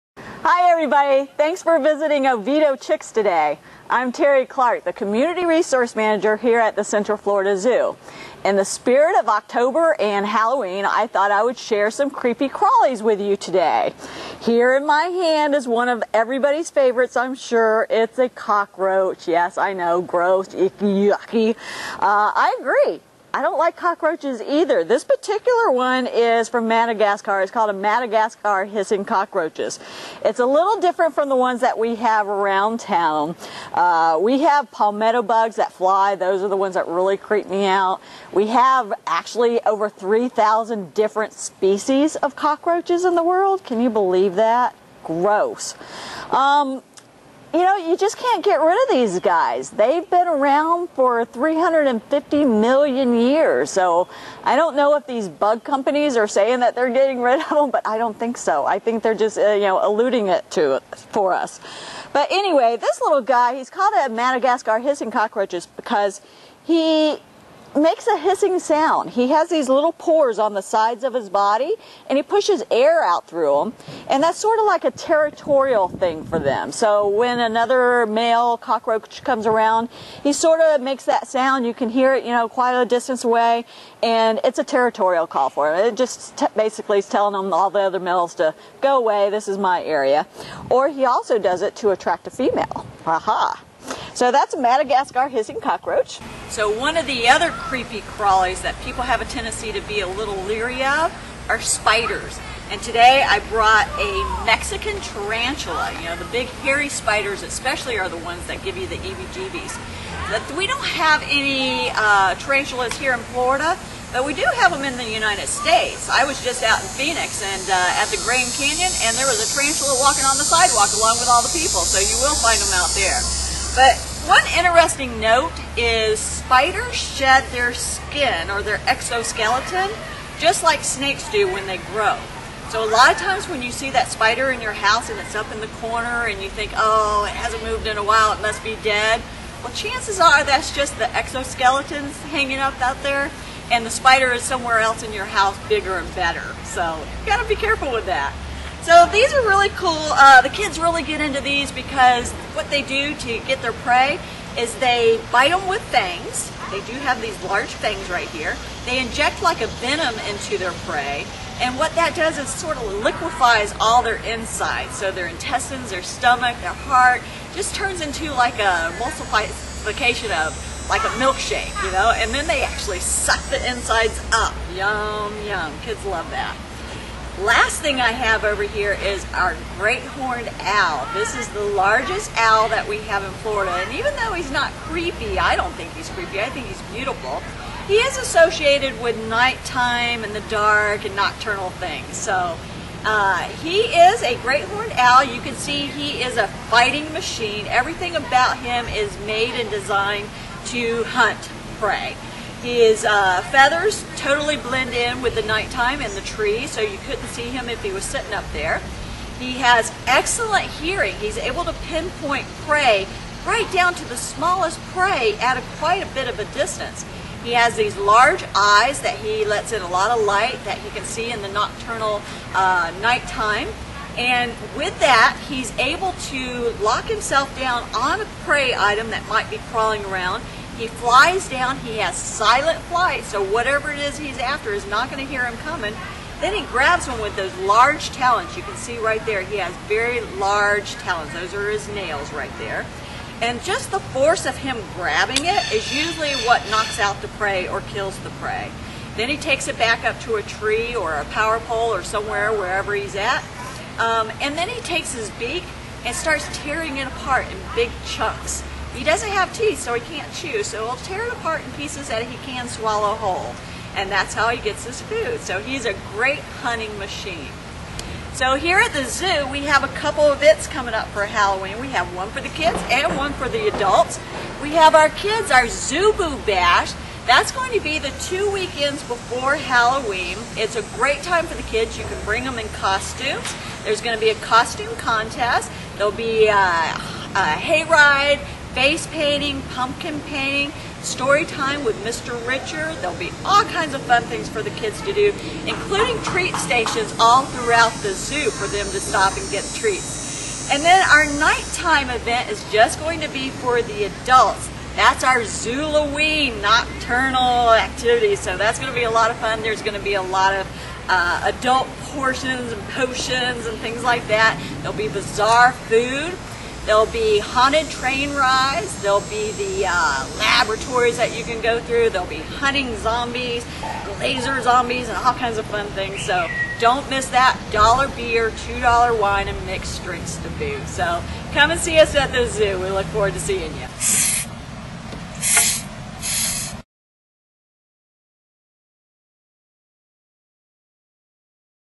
Halloween at the Zoo!
and Teri Clark from the Central Florida Zoo show you just how much fun you can have at the zoo on Halloween.  To learn more about events at the Central Florida Zoo please